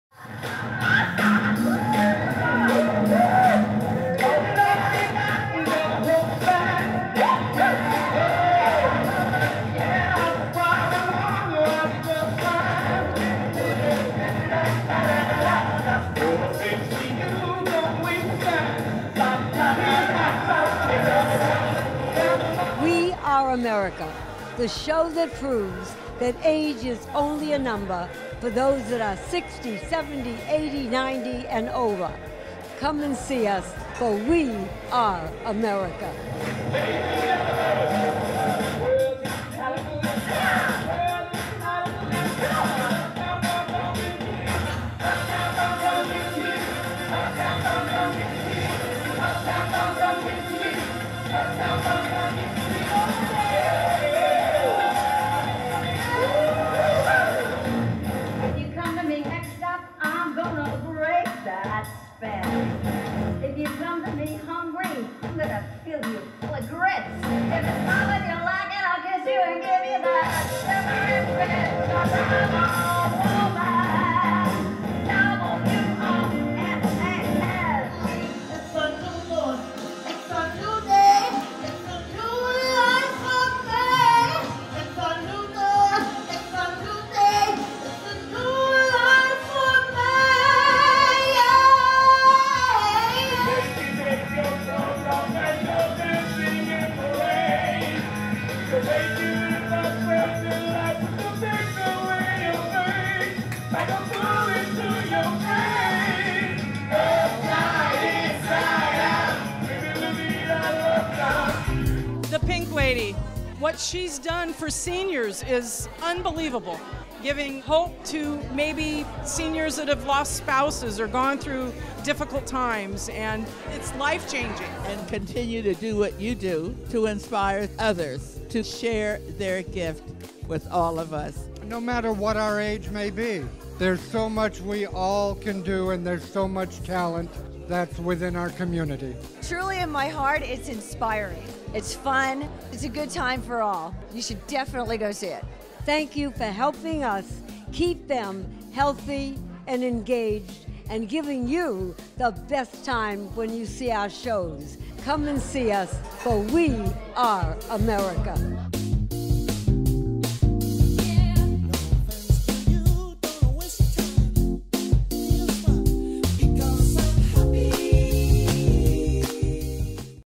"WE ARE AMERICA" SIZZLE REEL: Extended
Pink Lady presents "We Are America," a musical salute to our veterans!  All performers 60 years young & waaay over!

A Senior Star Power Production.